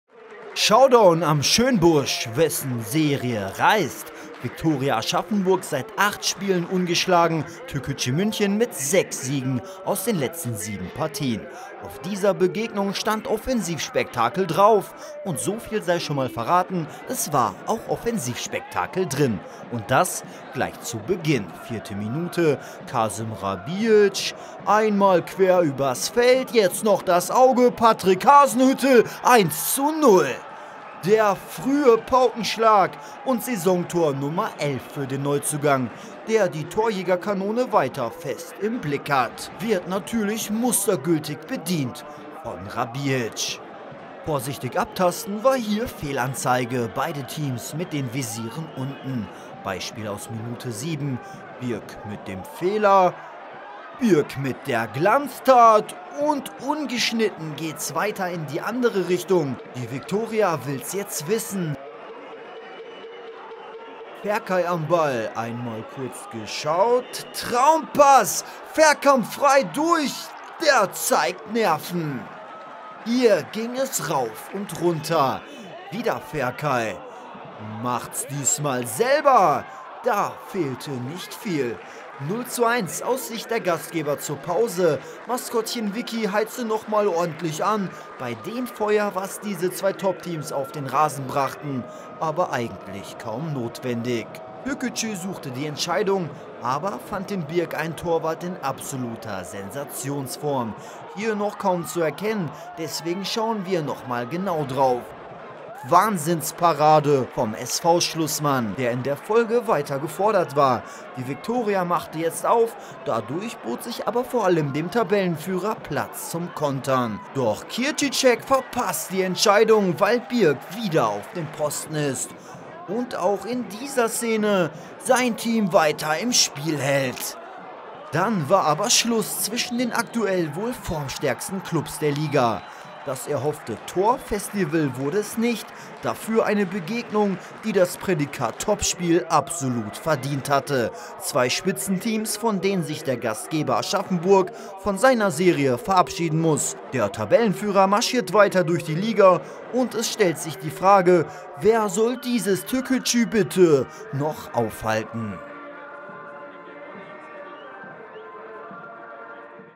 Zauberpässe, Sensations-Paraden, Offensiv-Spektakel! Aschaffenburg - Türkgücü München | BFV.TV
Saison 2019/20 Regionalliga Bayern - Spieltag 19